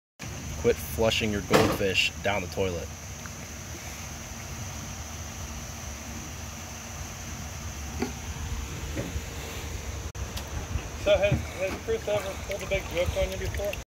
STOP FLUSHING YOUR PET GOLDFISH
Collected a goldfish on the Mad River (OH) during an electrofishing biological assessment. These evaluations are used to make determinations on water quality by studying the very organisms that live within the river, aka stream biological indicators. We study the community as a whole to draw insight as to  Whether or not water quality is meeting standards.

Goldfish are non native exotic introduced species should not be released Into the wild.  They  Are able to persist  and make it to natural waterways under such circumstances because they are considered a tolerant species. This means that they can withstand Exposure to various environmental stressors including Lotus all of oxygen, ammonia etc..

 This is kind of why they are such a popular pet.  Anyhow I want people to be conscious to do whatever they can to inhibit this from happening as much as possible.